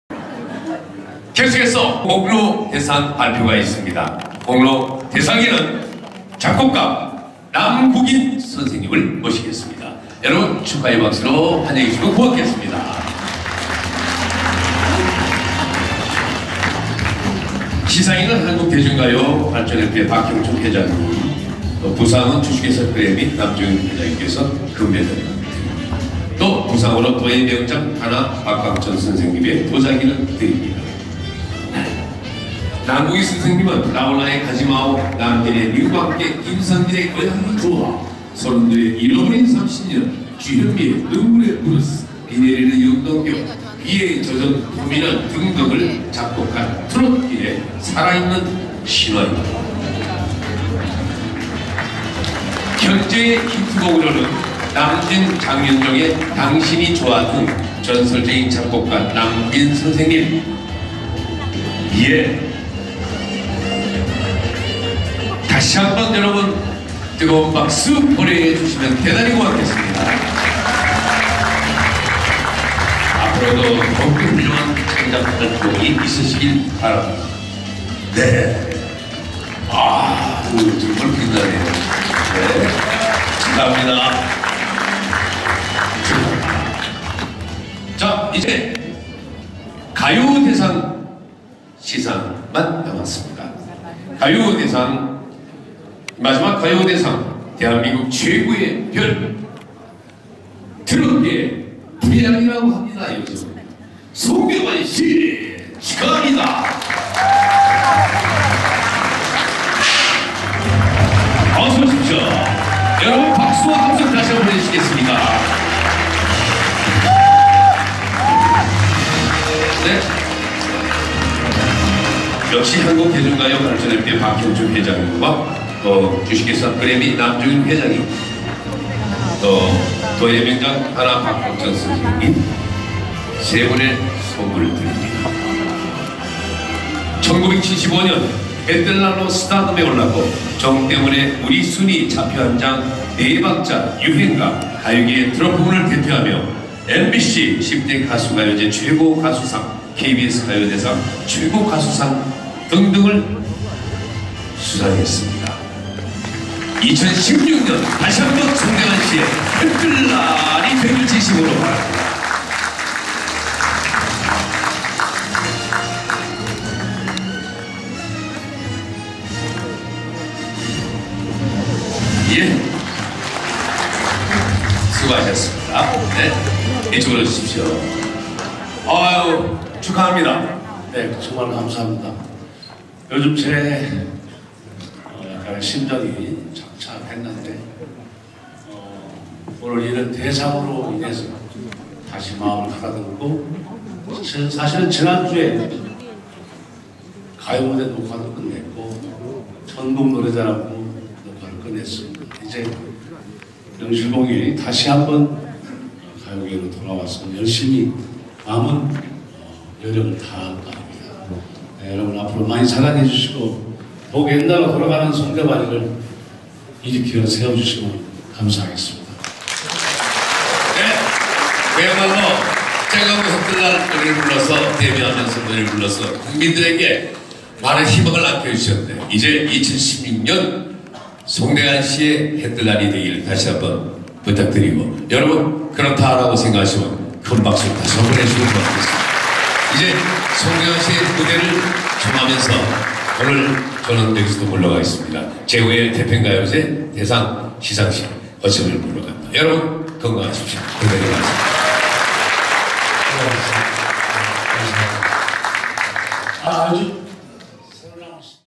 송대관, 제5회 가요대상 시상식,문화예술TV21 옥토배기 촬영편집 31
한국대중가요발전협회가 주최하는 제5회 대평가요대상 시상식이 1월7일 강원도 철원 남종현 센터에서 열렸다.

사회 허참 씨의 진행으로 열린 이번 행사에서 공로대상 외 여주도예명장 단아 박광천 명장 등 각각 공로상을 수상하였으며 또한 송대관 씨가 가요대상을 수상했으며 김용임, 진성, 박상철, 한혜진 가수는 최고인기상을 수상, 우연이, 이혜리씨는 인기상을 수상, 인기상에는 정정아, 현당 씨, 신인상은 정은, 전부상, 듀엣상에는 미스미스터, 가창상은 정하나, 진운, 윤완식과 특별상으로는 이범준, 이탁씨가 각각 수상하였다.

☞ 문화예술TV21 사업자번호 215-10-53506, 대표/편집인 김영재